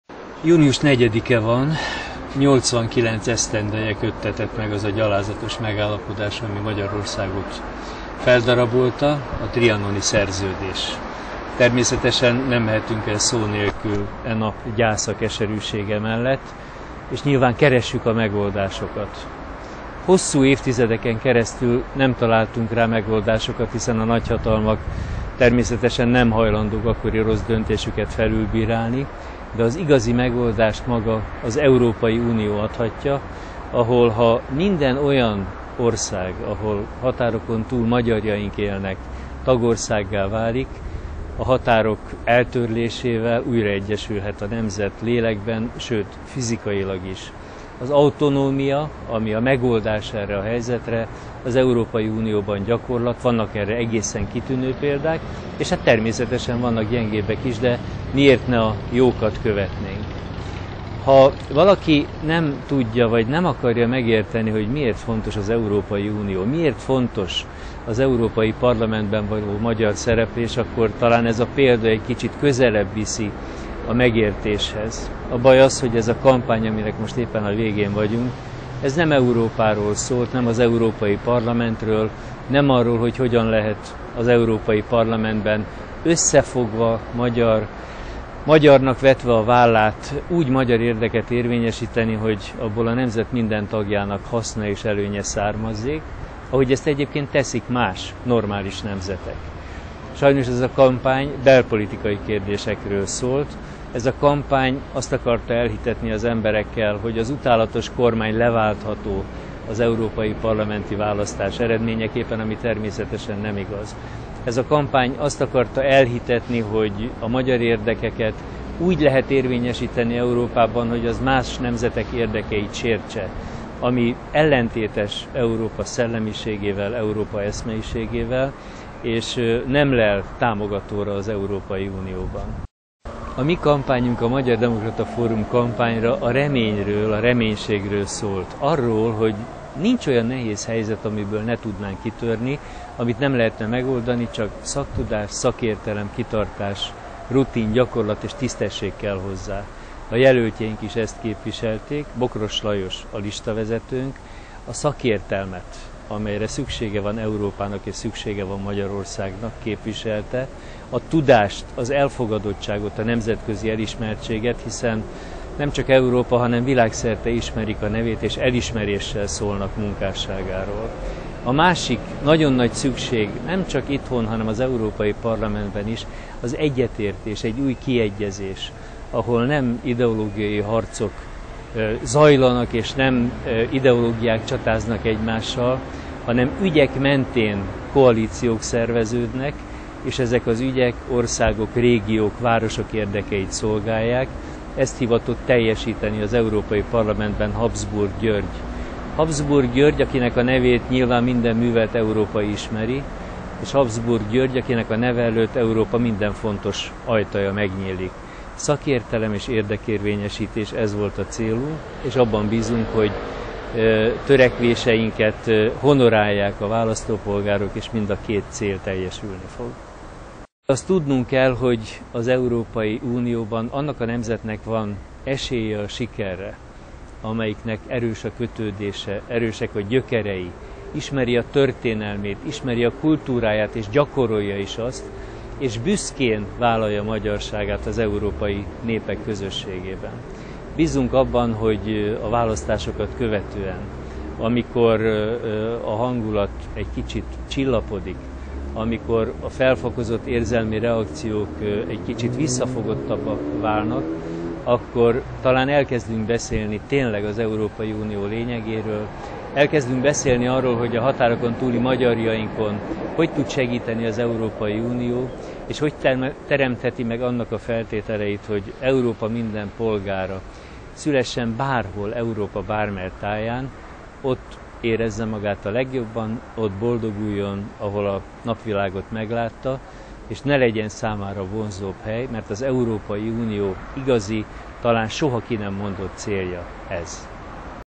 Herényi kampanyzáró videóblog
Herényi Károly az MDF alelnöke az EP-választásokról